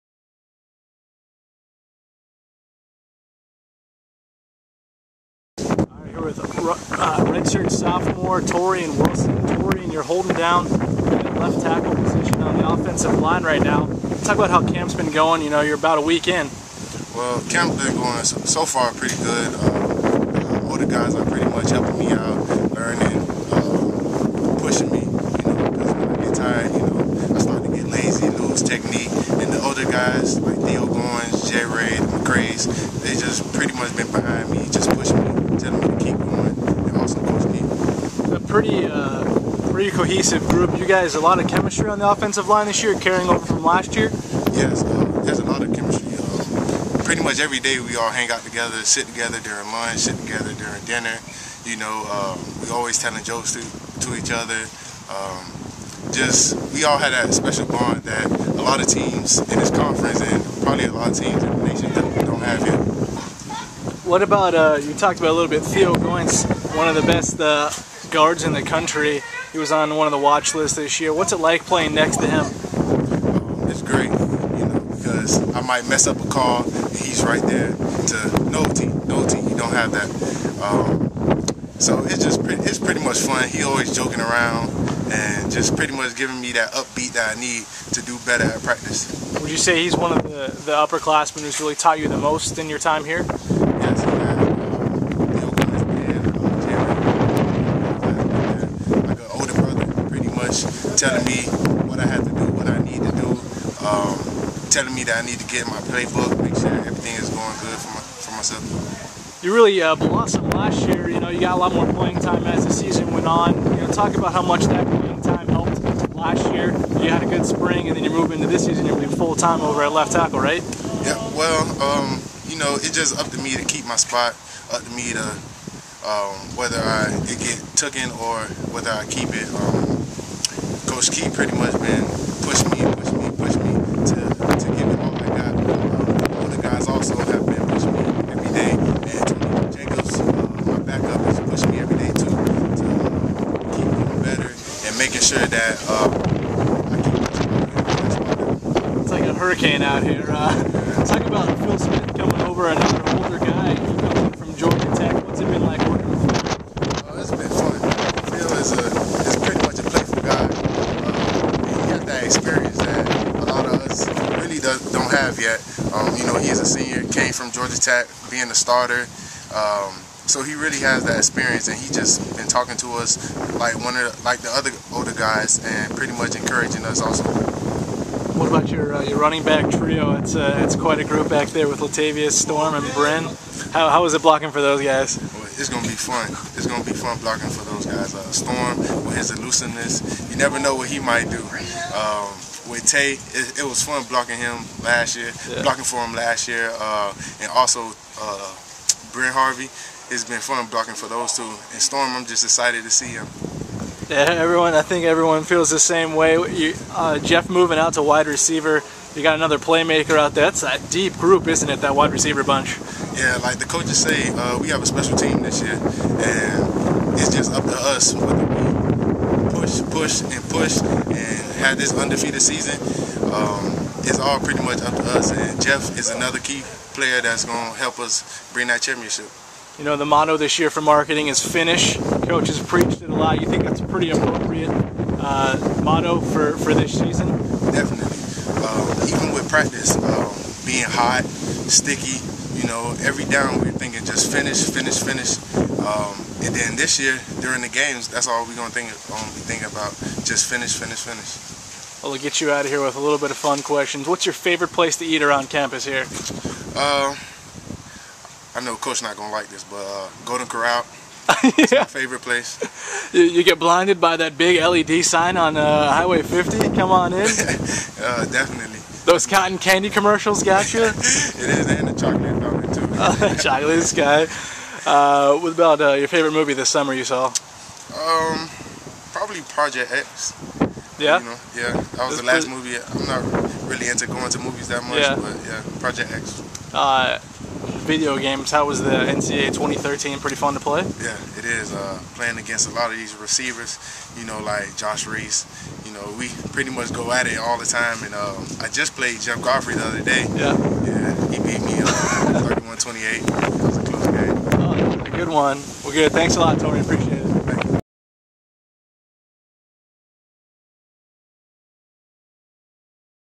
UCF OL Torrian Wilson Breaks Down Fall Camp (8.7.12)
UCF offensive lineman Torrian Wilson is entering his redshirt sophomore season. In his interview, the Miami native breaks down how the offensive line is progressing, the Knights' motto of "Finish" and his favorite restaurant around campus.